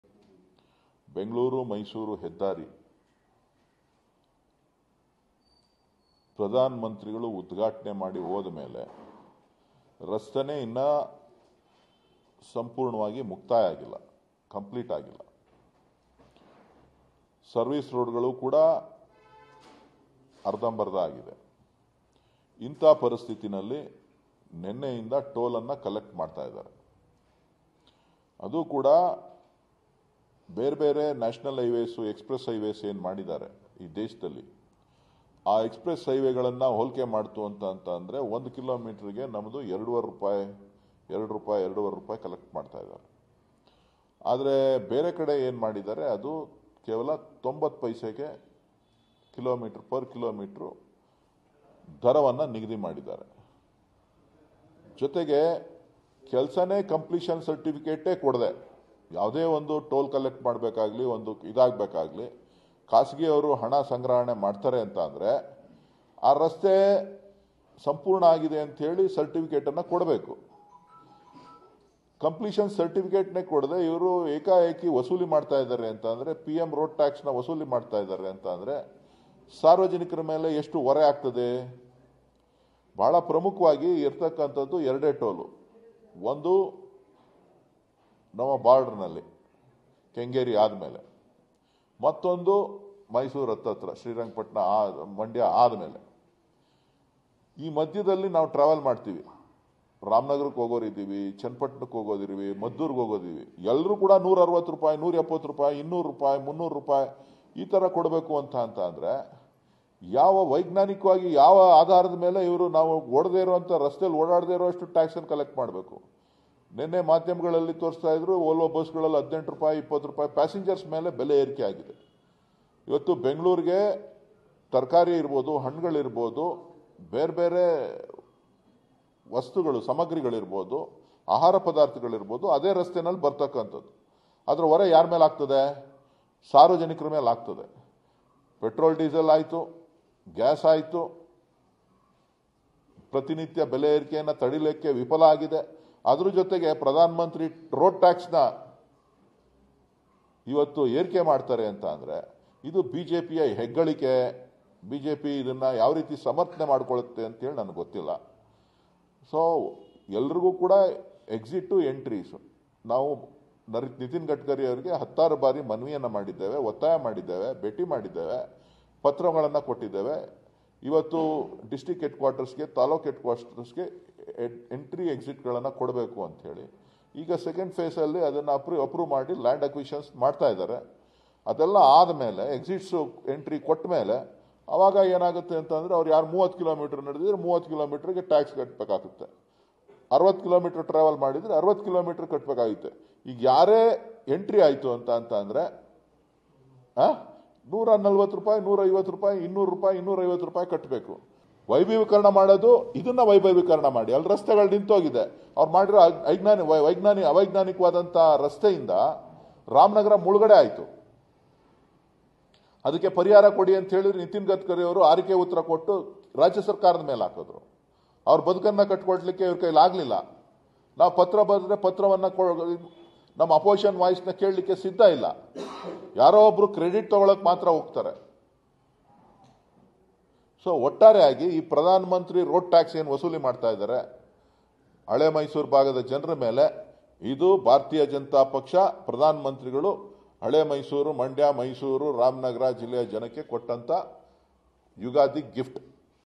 ಬೆಂಗಳೂರು- ಮೈಸೂರು ಹೆದ್ದಾರಿ ಟೋಲ್ ವಿಚಾರಕ್ಕೆ ಗರಂ ಆದ ಡಿಕೆ ಸುರೇಶ್ |DK Suresh
ಬೆಂಗಳೂರು ಮೈಸೂರು ಎಕ್ಸ್ ಪ್ರೆಸ್ ವೇ ನಲ್ಲಿ
ರಸ್ತೆ ಕಾಮಗಾರಿ ಪೂರ್ತಿಯಾಗಿಲ್ಲದಿದ್ದರೂ ಟೋಲ್ ಕಲೆಕ್ಟ್ ಮಾಡ್ತಿದ್ದಾರೆ
ಒಂದು ಕಿಮಿ.ಗೆ ೨ ರಿಂದ ೩ ರೂ ಕಲೆಕ್ಟ್ ಮಾಡ್ತಿದ್ದಾರೆ
ಟೋಲ್ ಕಲೆಕ್ಟ್ ಮಾಡಬೇಕಾದ್ರೆ ರೋಡ್ ಕಂಪ್ಲಿಷನ್ ಸರ್ಟಿಫಿಕೇಟ್ ಕೊಡಬೇಕಾಗುತ್ತದೆ
ಆದ್ರೆ ರಸ್ತೆ ಕಂಪ್ಲಿಟ್ ಆಗದೆ ಸರ್ಕಾರ ದುಡ್ಡು ವಸೂಲಿ ಮಾಡ್ತಿದ್ದಾರೆ
ಎರಡೆ ಟೋಲ್‌ಗಳಿದೆ ಬಿಡದಿ,ರಾಮನಗರ, ಮದ್ದೂರು,ಮಂಡ್ಯಕ್ಕೂ ಹೋಗೊಕೆ ಎಲ್ಲರಿಗೂ ಒಂದೇ ದುಡ್ಡು ಅಂದ್ರೆ ಹೇಗೆ
ಕೆ ಎಸ್ ಆರ್ ಟಿ ಸಿ ಬಸ್ ದರ ಕೂಡ ಹೆಚ್ಚಳ ಮಾಡೋದಾಗಿ ಹೇಳಿದ್ದಾರೆ
ಹಣ್ಣು,ತರಕಾರಿ ಎಲ್ಲವೂ ಆ ಮೂಲಕವರ ಬರಬೇಕು
ಹೀಗೆ ಹೆಚ್ಚಳ ಮಾಡೋದ್ರಿಂದ ಪೂರಕ ವಸ್ತುಗಳ ಬೆಲೆ ಹೆಚ್ಚಳವಾಗುತ್ತದೆ ಜನರಿಗೆ ಹೊರೆ ಆಗುತ್ತದೆ
ಅನೇಕ ಬಾರಿ ನಾನು ನಿತಿನ್ ಗಡ್ಕರಿ ಅವರ ಜೊತೆ ಕೂಡ ಮಾತನಾಡ್ತೀನಿ
ಆದ್ರೆ ಟೋಲ್ ಕಲೆಕ್ಟ್ ಮಾತ್ರ ಅವೈಜ್ಞಾನಿಕವಾಗಿ ನಡೆಯುತ್ತಿದೆ
ಪಿಎಂ ಟ್ಯಾಕ್ಸ್ ಪಿಎಂ ಟೋಲ್  ಬಿಜೆಪಿ ಹಳೇಮೈಸೂರು ಭಾಗದ ಜನರಿಗೆ ಕೊಟ್ಟ ಯುಗಾದಿ ಗಿಫ್ಟ್